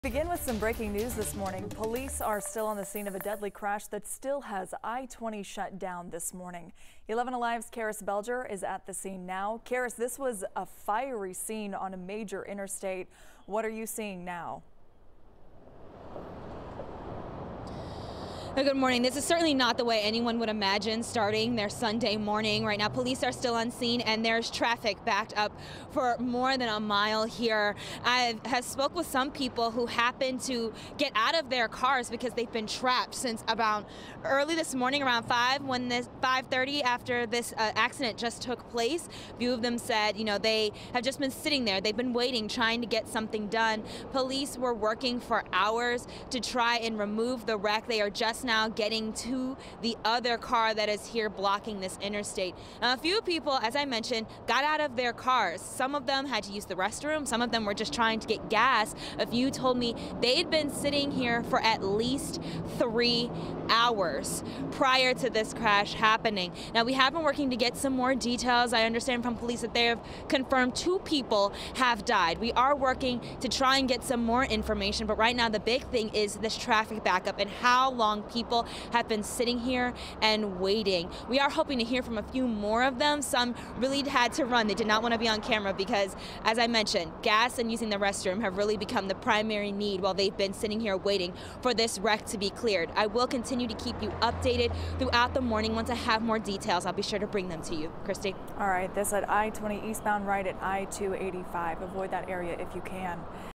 Fiery wreck on I-20 leaves 2 dead in Atlanta
It happened just before 5:30 a.m. on Sunday.